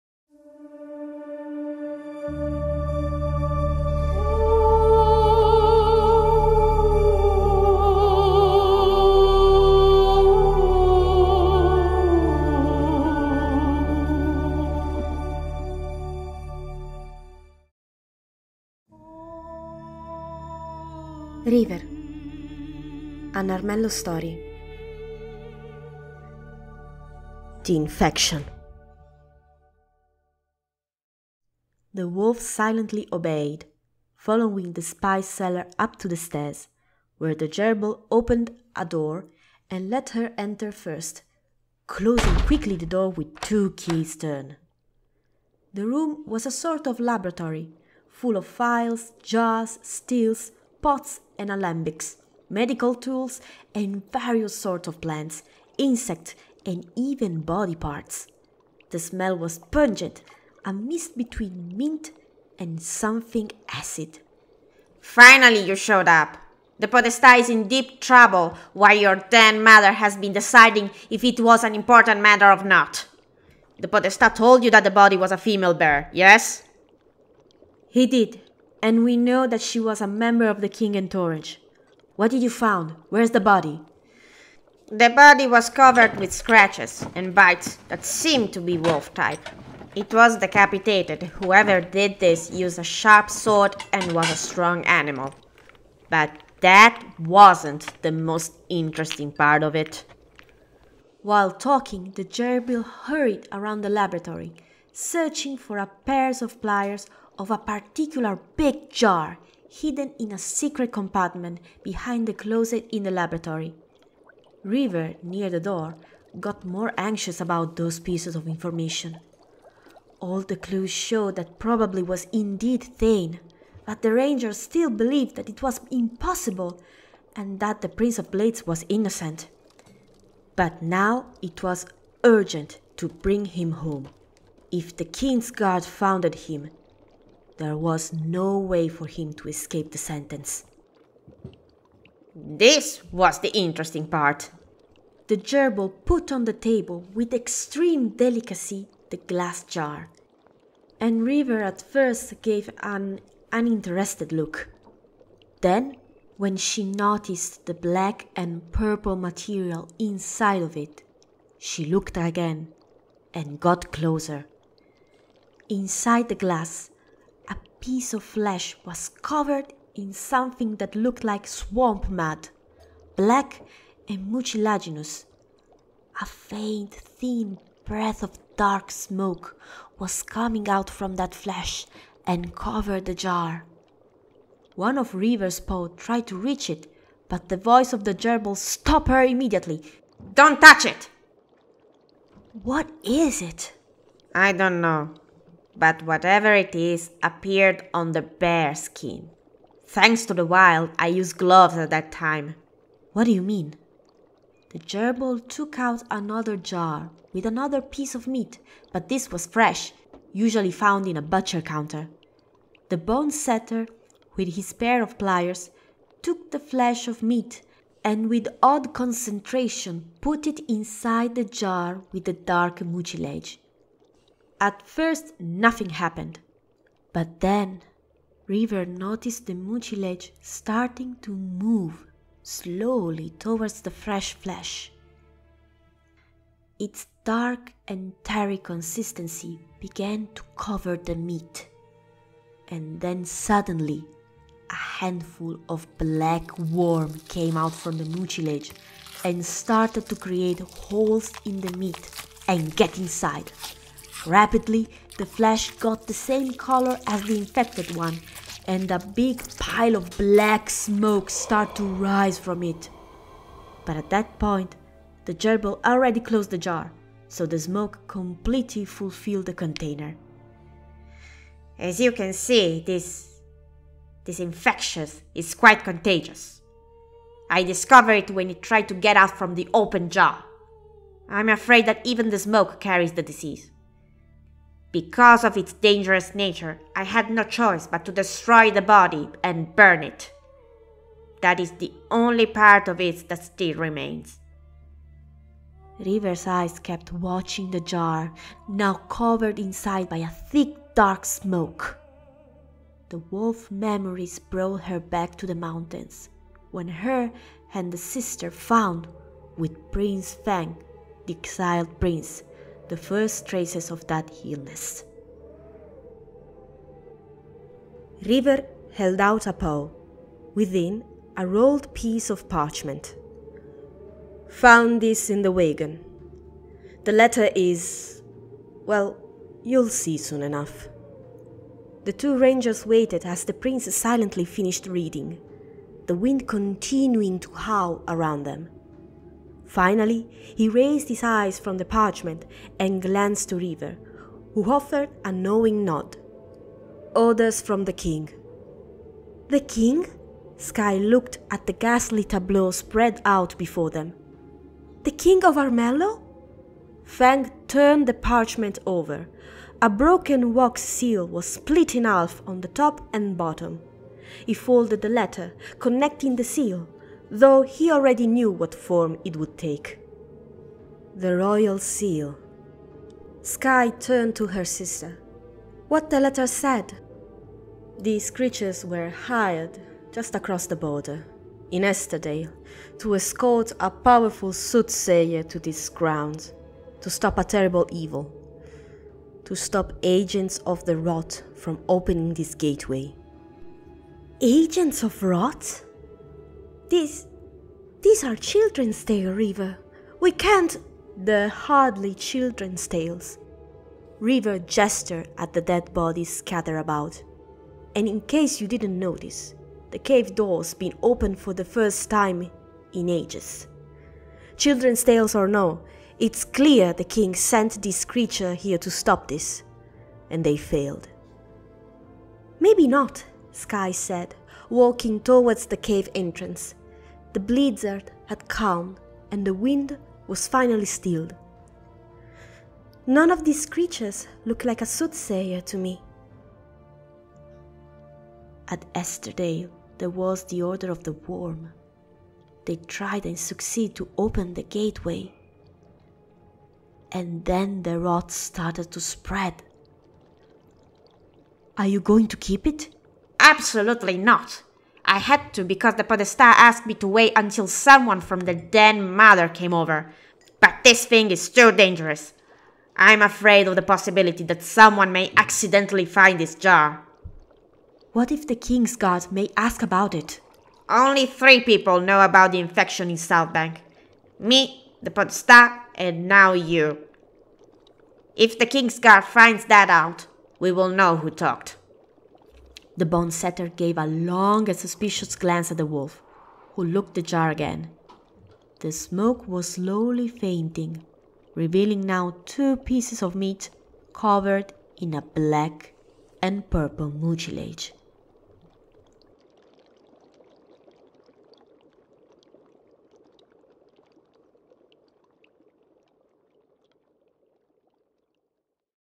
RIVER, AN ARMELLO STORY [Chapter 4]: The Infection
The wolf silently obeyed, following the spice seller up to the stairs, where the gerbil opened a door and let her enter first, closing quickly the door with two key turns.
La lupa obbedì silenziosamente, seguendo il venditore di spezie fino alle scale, dove il gerbillo aprì una porta e la fece entrare per prima, chiudendo velocemente la porta con due giri di chiave.